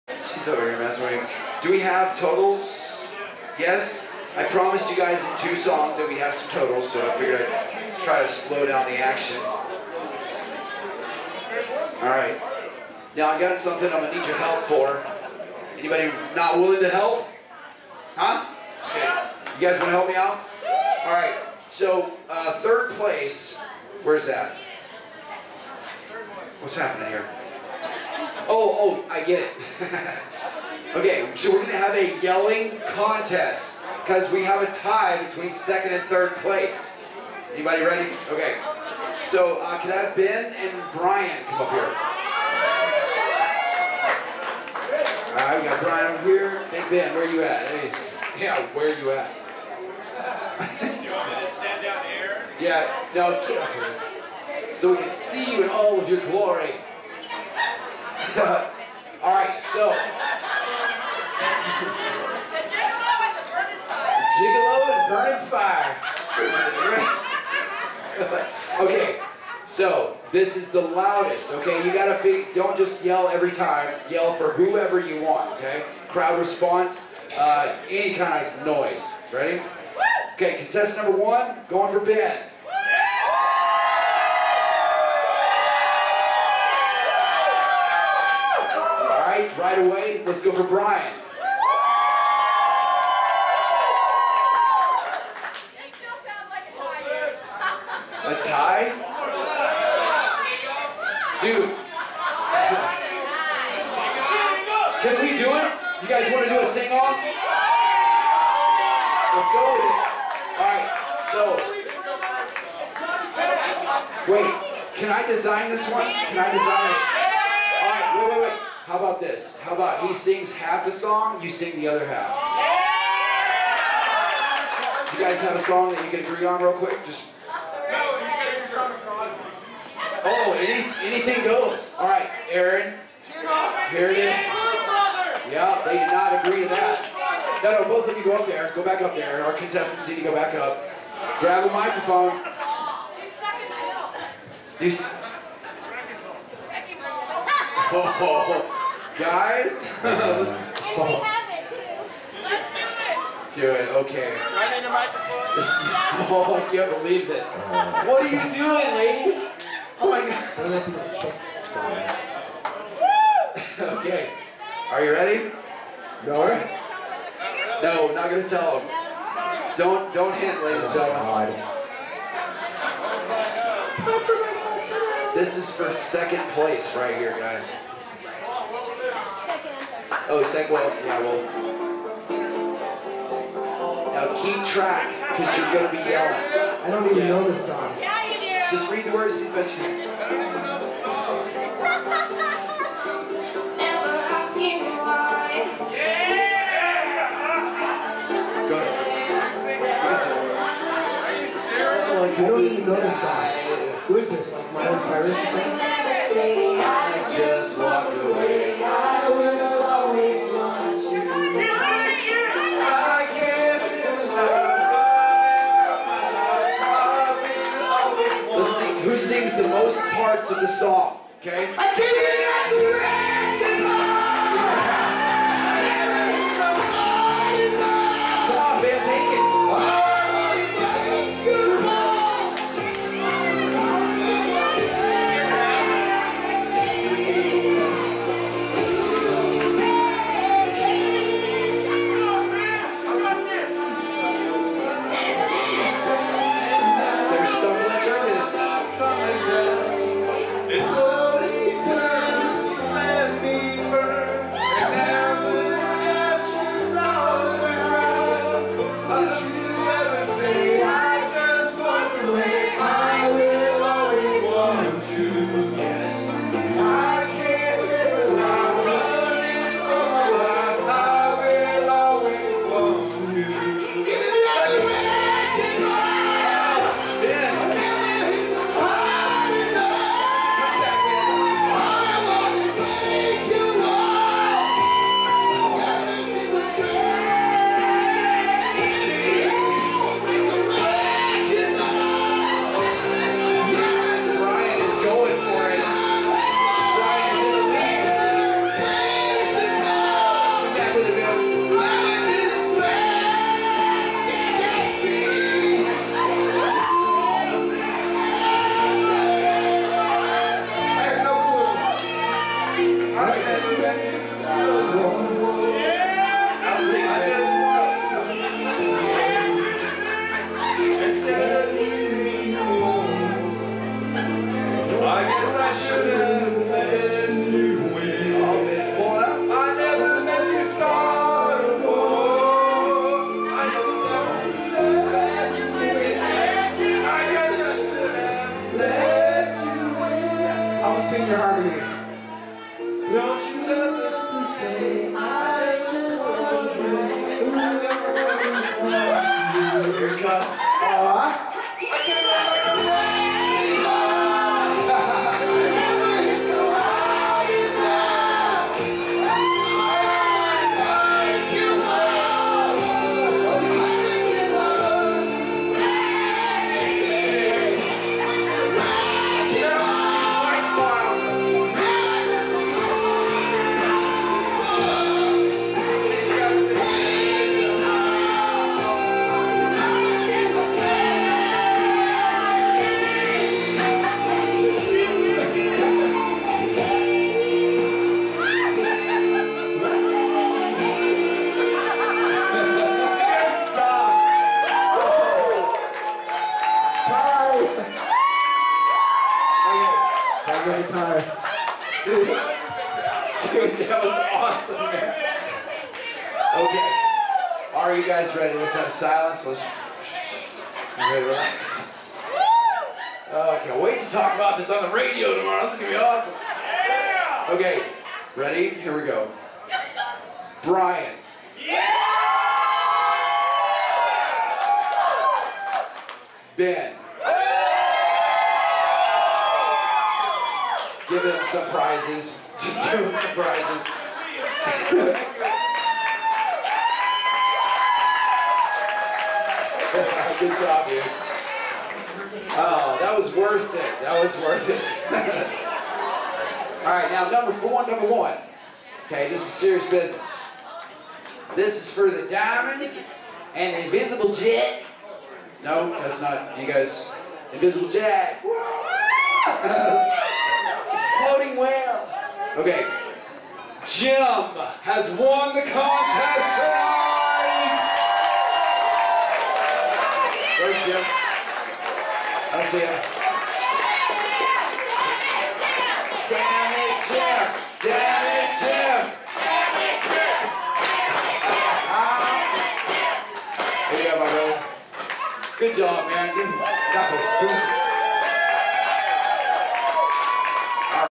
31ContestTieandResults
Last night of karaoke with Dr Jack at Three Rivers Casino and Hotel. Thanks to everyone who made this a great time.